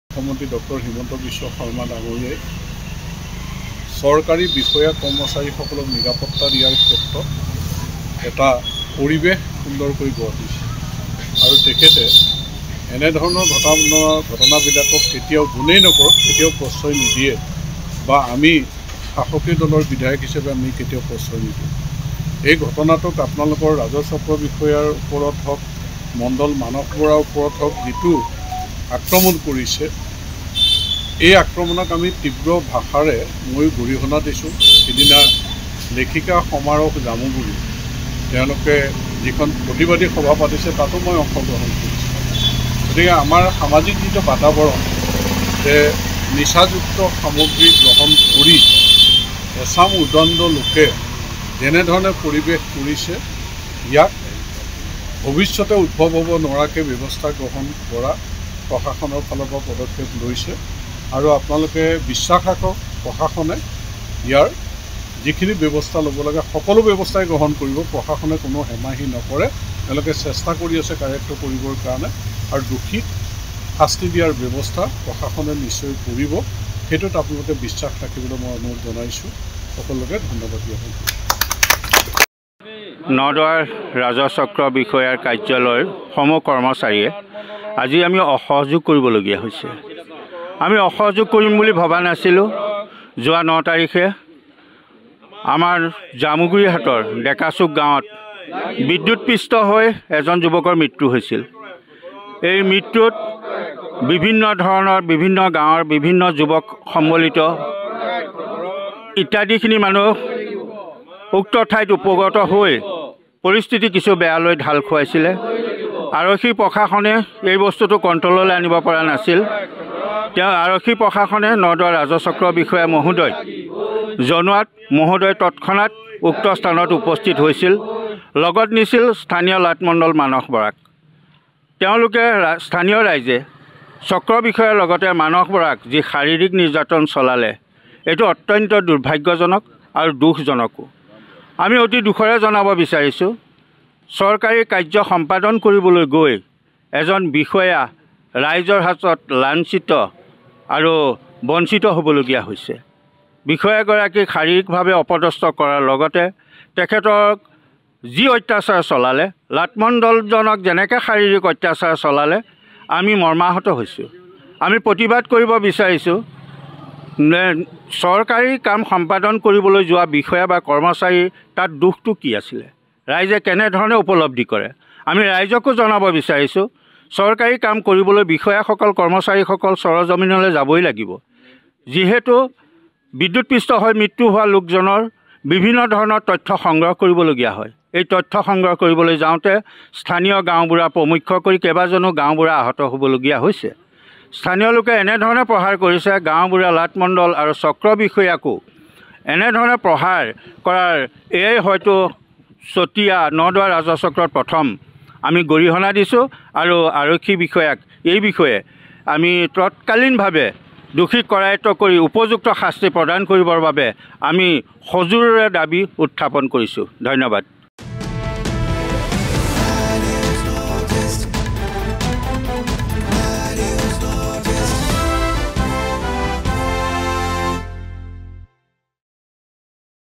নদুৱাৰ ৰাজহ চক্ৰ বিষয়াৰ কাৰ্যালয়ৰ কৰ্মচাৰীৰ অসহযোগ ধৰ্মঘট কাৰ্যসূচী
My News NE:  নদুৱাৰ ৰাজহ চক্ৰ বিষয়াৰ কাৰ্যালয়ৰ কৰ্মচাৰীৰ অসহযোগ ধৰ্মঘট কাৰ্যসূচী